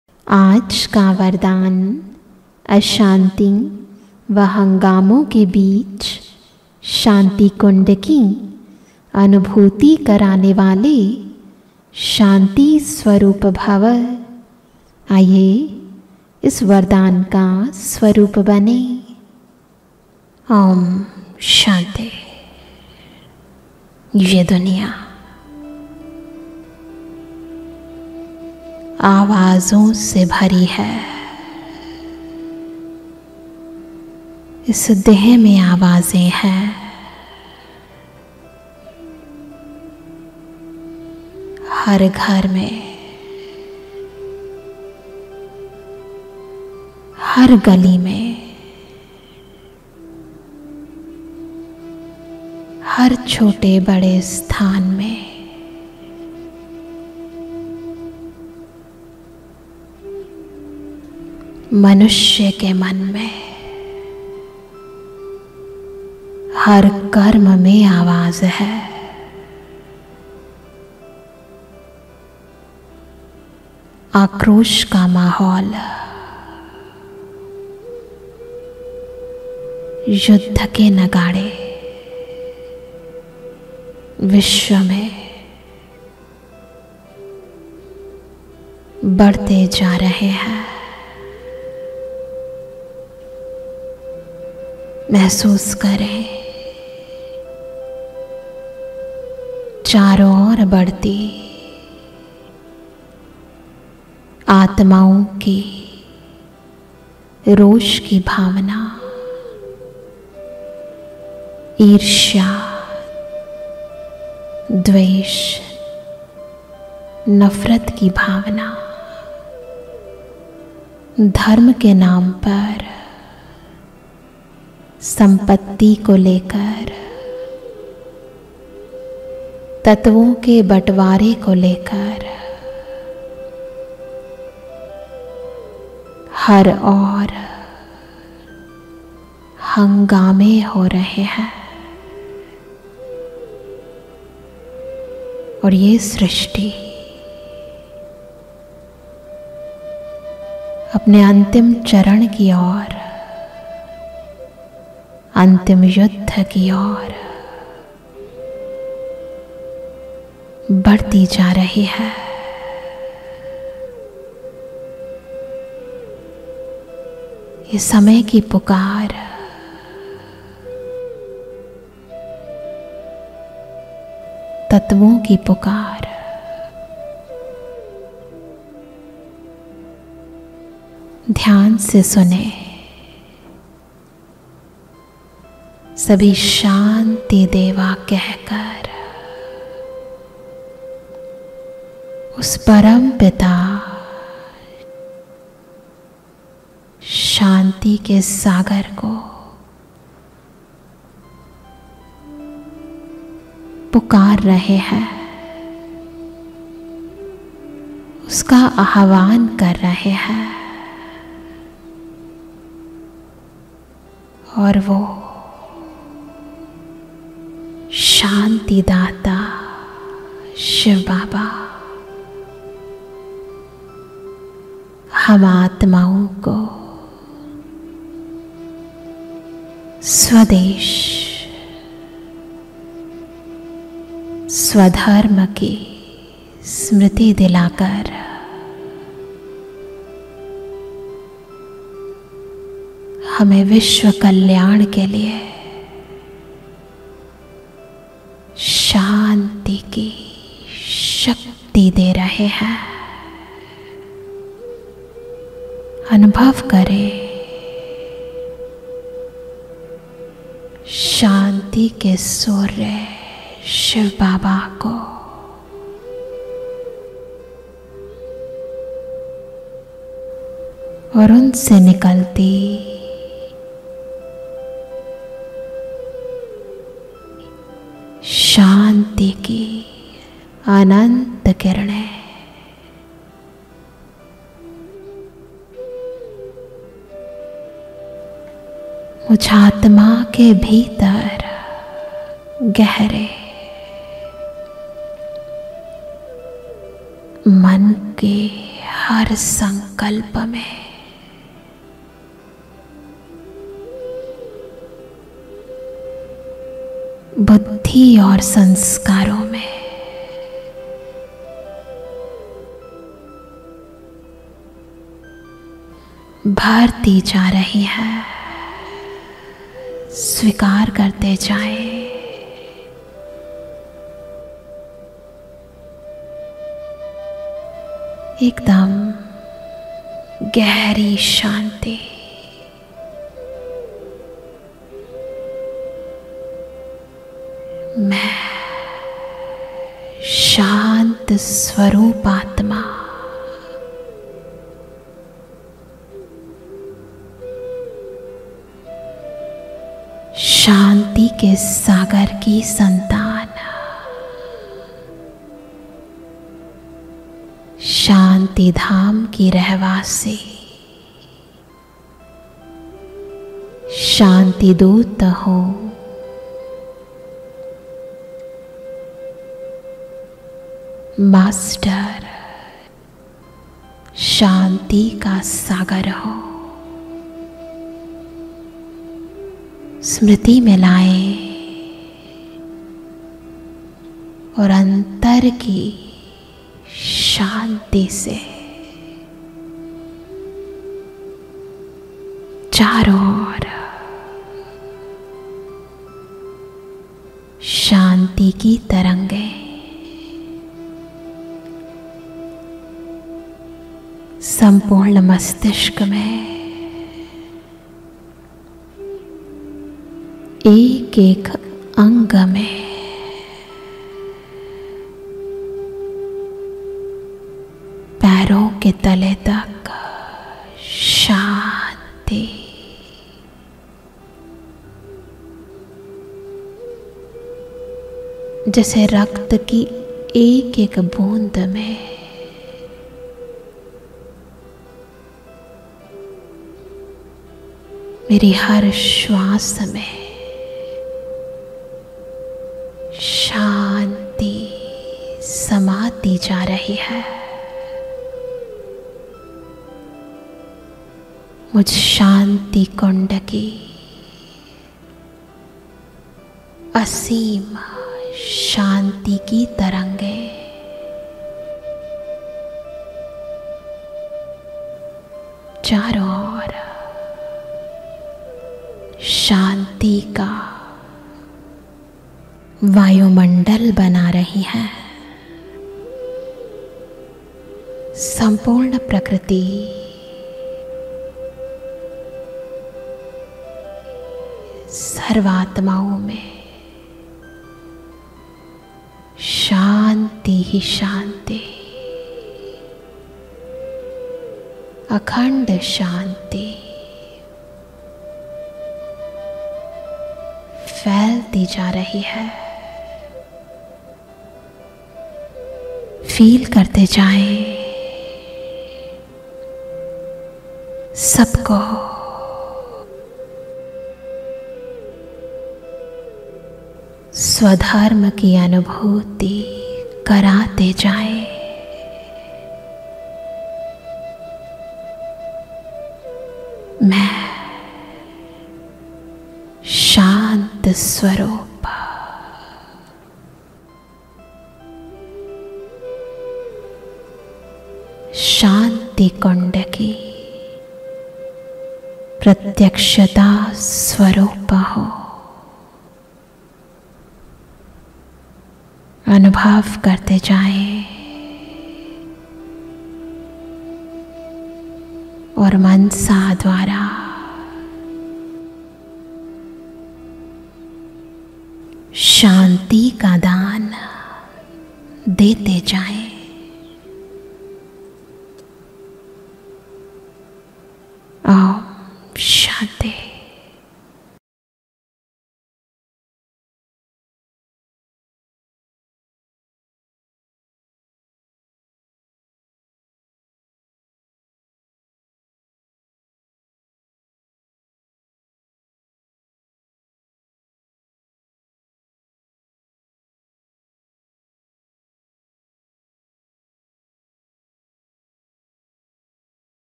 AAJ KA VARDAN -25-07-2023, BK ADITI, RAJYOGA TEACHER, GYANSAROVAR, MOUNT ABU, RAJASTHAN, INDIA.
आज का वरदान - शांति की शक्ति फैलाने का अभ्यास
GUIDED MEDITATION COMMENTARY.
GODS BLESSINGS. MEDITATION COMMENTARY. ME AND GOD.
SPIRITUAL AWAKENING.SPIRITUALITY.
SHANTI KI SHAKTI FAILANE KA ABHYAS.
CATEGORY  :  EDUCATION